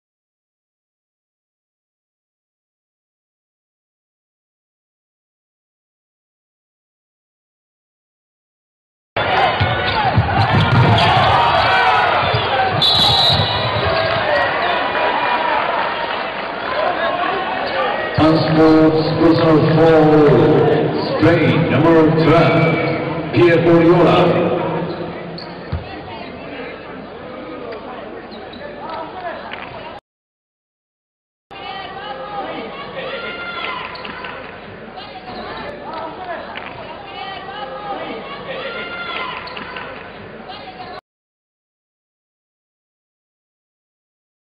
REFSTYLE: WHAT IS YOUR DECISION... CLIP 437
Art. 37 Unsportsmanlike foul
37.1 Definition
37.1.1 An unsportsmanlike foul is a player contact which, in the judgement of a referee is:
• Contact with an opponent and not legitimately attempting to directly play the ball within the spirit and intent of the rules.
• Excessive, hard contact caused by a player in an effort to play the ball or an opponent.
• An unnecessary contact caused by the defensive player in order to stop the progress of the offensive team in transition. This applies until the offensive player  begins his act of shooting.
• An illegal contact caused by the player from behind or laterally on an opponent, who is progressing towards the opponent’s basket and there are no other players between the progressing player, the ball and the basket. This applies until the offensive player begins his act of shooting.
• Contact by the defensive player on an opponent on the playing court when the game clock shows 2:00 minutes or less in the fourth quarter and in each overtime, when the ball is out-of-bounds for a throw-in and still in the hands of the referee or at the disposal of the player taking the throw-in.
37.1.2 The referee must interpret the unsportsmanlike fouls consistently throughout the game and to judge only the action.
37.2 Penalty
37.2.1 An unsportsmanlike foul shall be charged on the offender.
37.2.2 Free throw(s) shall be awarded to the player who was fouled, followed by:
• A throw-in from the throw-in line in the team’s frontcourt.
• A jump ball in the centre circle at the beginning of the first quarter.
The number of free throws shall be awarded as follows:
• If the foul is committed on a player not in the act of shooting: 2 free throws.
• If the foul is committed on a player in the act of shooting: the goal, if made, shall count and, in addition, 1 free throw.
• If the foul is committed on a player in the act of shooting and the goal is not made, 2 or 3 free throws.
37.2.3 A player shall be disqualified for the remainder of the game when he is charged with 2 unsportsmanlike fouls or 2 technical fouls, or with 1 technical foul and 1 unsportsmanlike foul.
37.2.4 If a player is disqualified under Art. 37.2.3, that unsportsmanlike foul shall be the only foul to be penalised